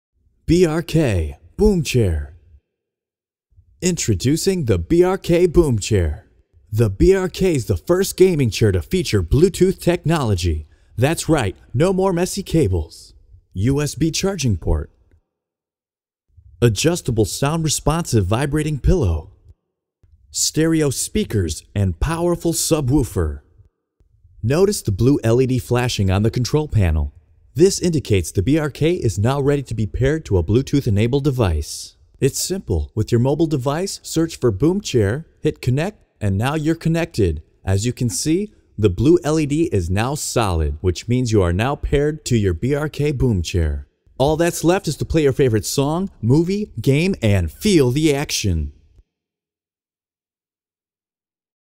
Boomchair Gaming Chair for Tablets, Gaming and Movies
Video transcript: BRK Boomchair.

Introducing the BRK Boomchair. The BRK is the first gaming chair to feature Bluetooth technology, that’s right no more messy cables. A USB chagrining port. Adjustable sound responsive vibrating pillow. Stereo speakers and powerful subwoofer. Notice the blue LED flashing on the control panel. This indicates that the BRK is ready to be paired to a Bluetooth enabled device. It’s simple. With your mobile device search for Boomchair, hit connect and now you’re connected. As you can see the blue LED is now solid which means you are now paired to your BRK Boomchair. All that’s left is to play your favorite song, movie or game and feel the action.